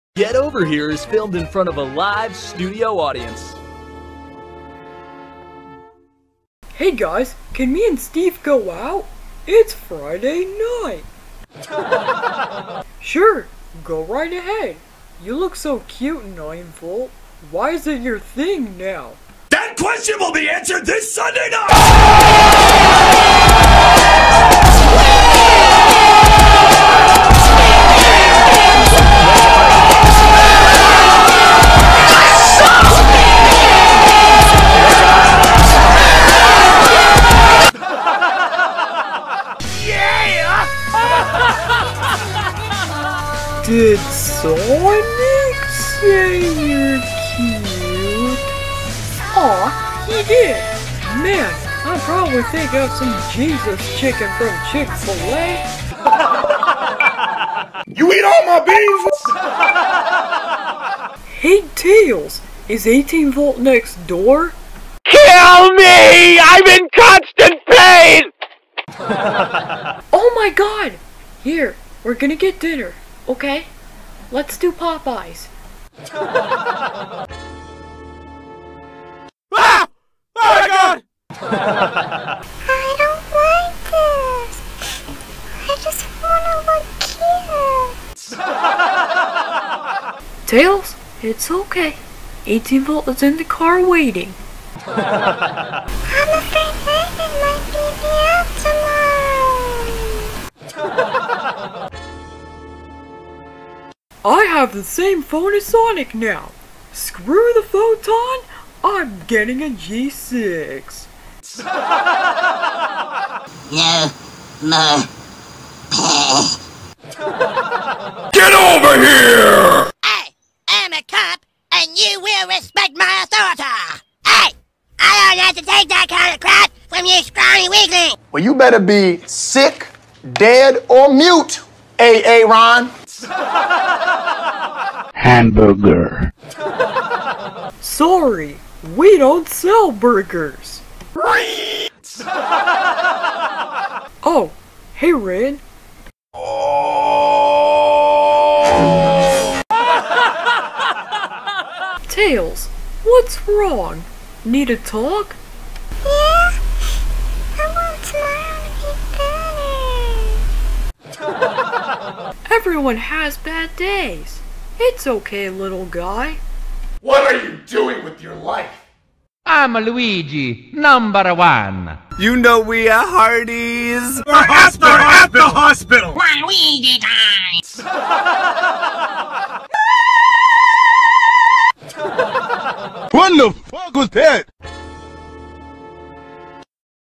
VGW Sitcom Movie: The Epic Friday
9-Volt and Steve are going to Popeyes